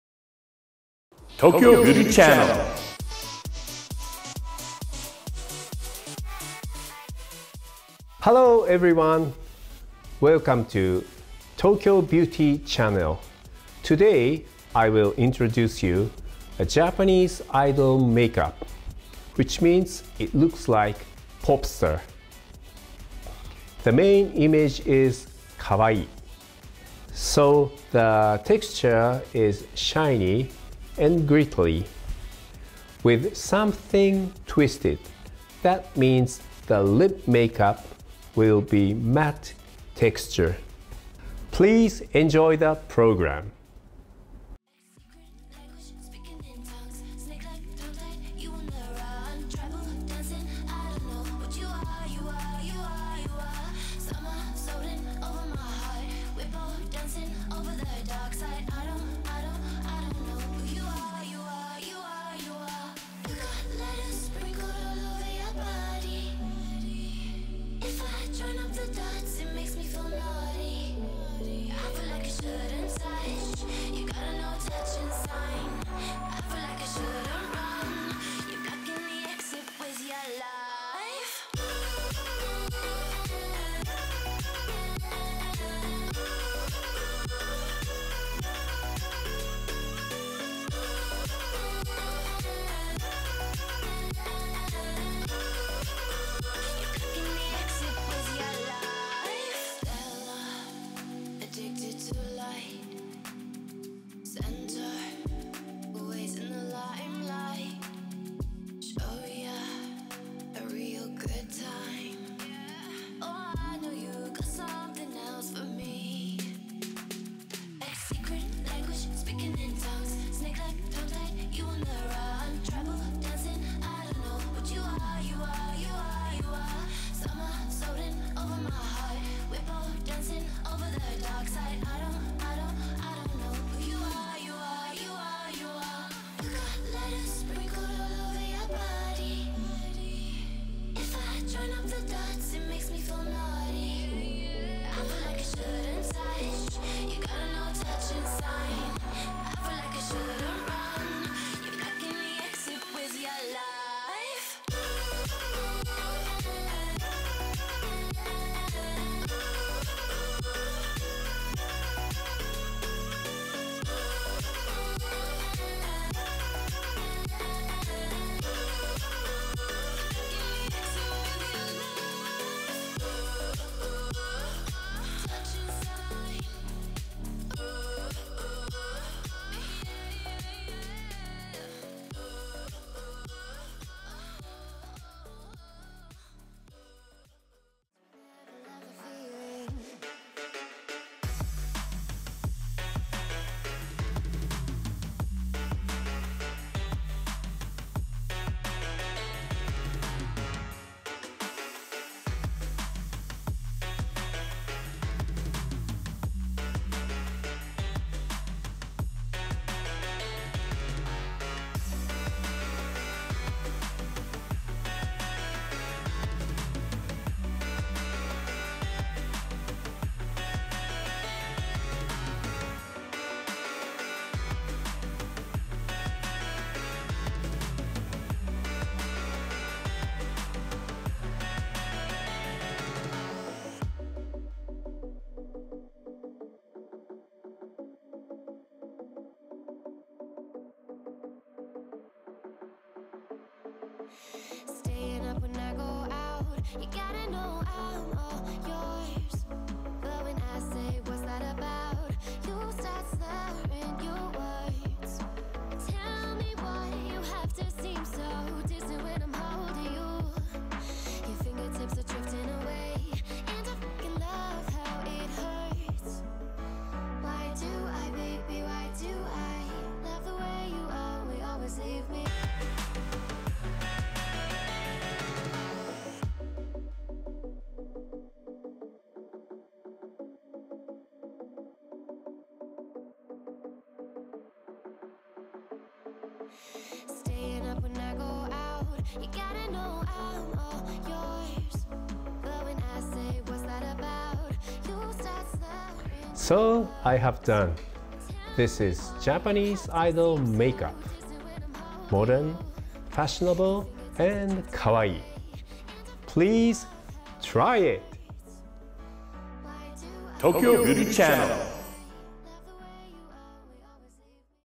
日本偶像化妝第２部分
This channel is makeup content by  Japanese makeup artist Hitomi NATORI

This thema is Japanese Idol makeup Part1

Model:Yuuki KUDO

Makeup artist: Hitomi NATORI

Hair make: Kiriko KASUMI

Camera: Yoshihiro KIKAWA, Shinya IINO

Narration: Michael O'Connor

Produce: Aim Production Inc.